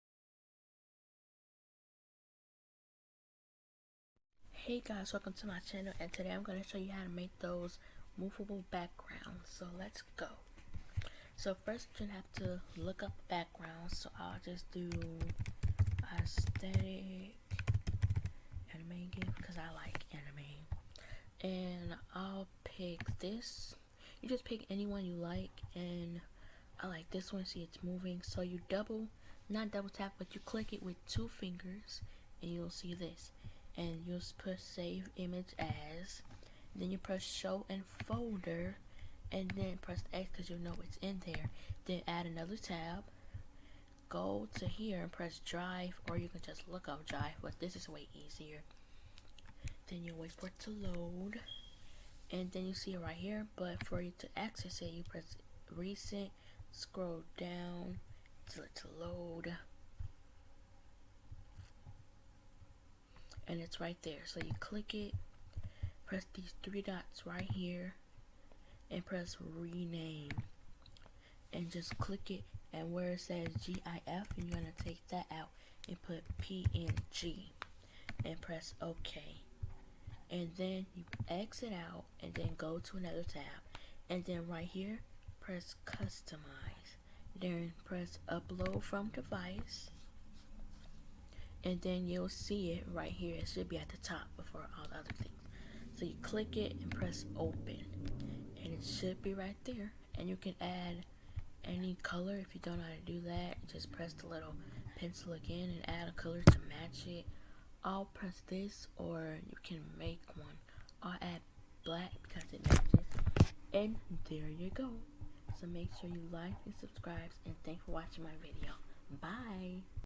How to make moveable background for computer (gif)
Heyy guys! First vid! Sry if My voice Is low-
Follow my socialsss
Byeeee :P